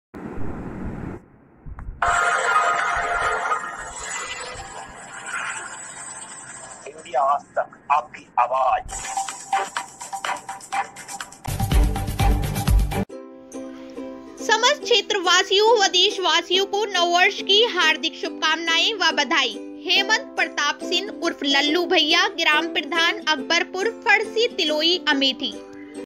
देशवासियों को नव वर्ष की हार्दिक शुभकामनाएं वह बधाई हेमंत प्रताप सिंह उर्फ लल्लू भैया ग्राम
समस्त क्षेत्रवासियों व देशवासियों को नव वर्ष की हार्दिक शुभकामनाएं वह बधाई

हेमंत प्रताप सिंह उर्फ लल्लू भैया ग्राम प्रधान अकबरपुर फरसी तिलोई अमेठी